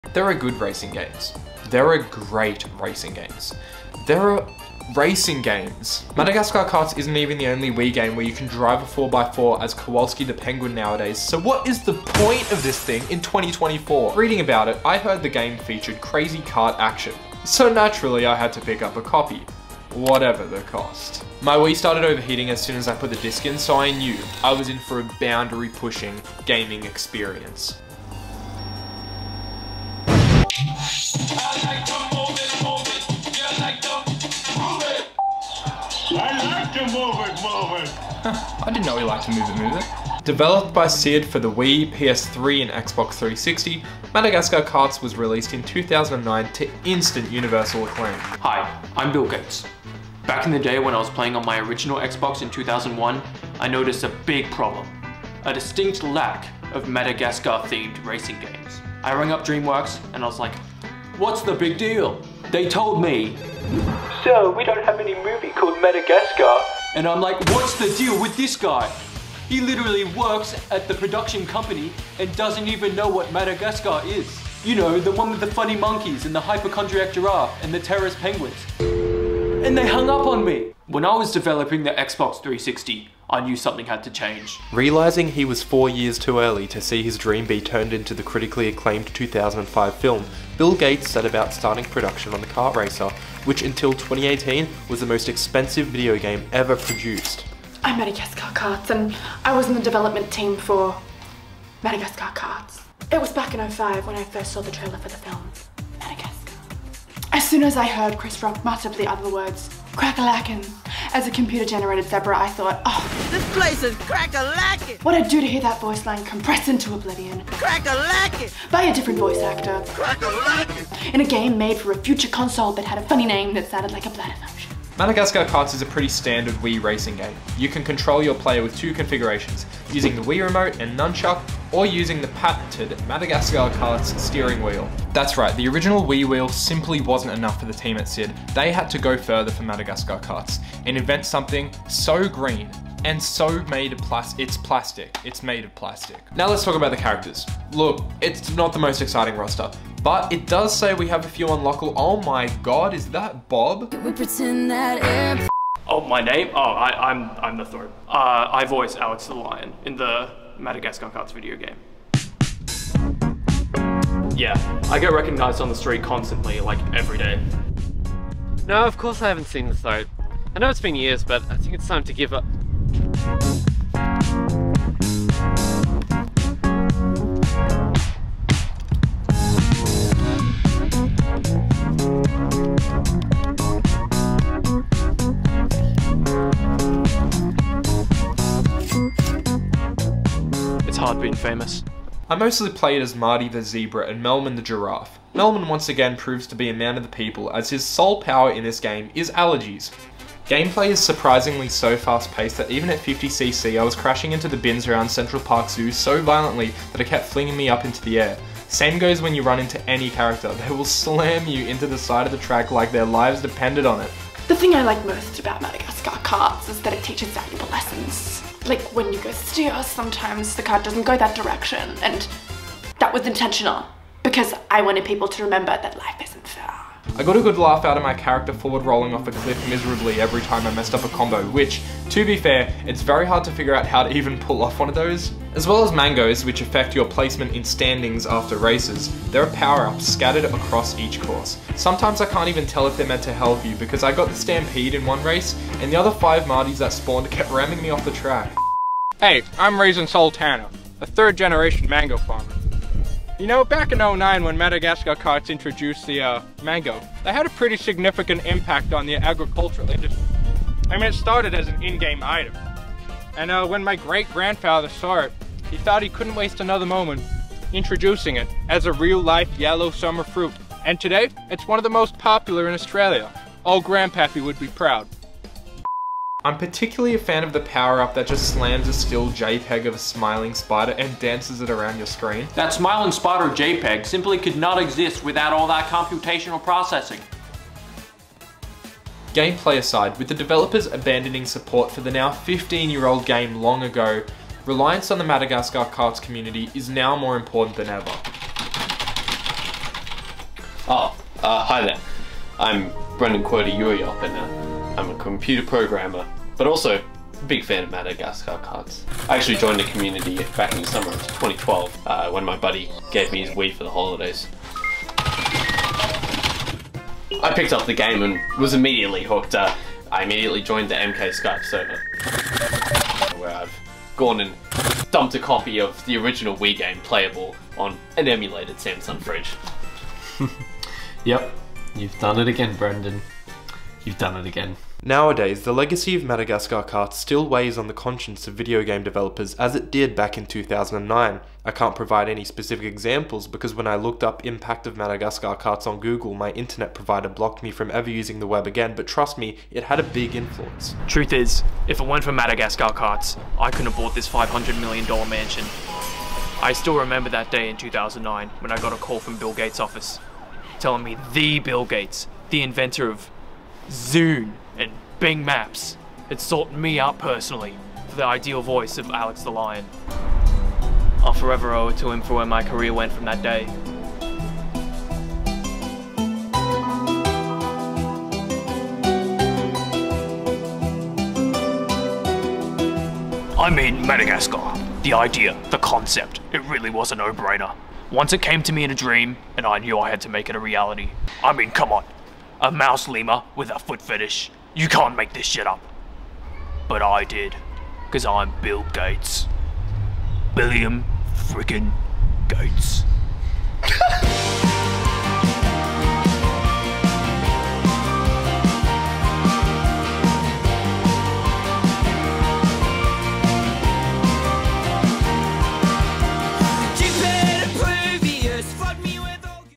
The formidable legacy of Madagascar Kartz
I take a look at Madagascar Kartz, everyone's second favourite Wii racing game that involves Madagascar characters.

Thanks to Lucas, Laura and Ziggy for appearing in this one.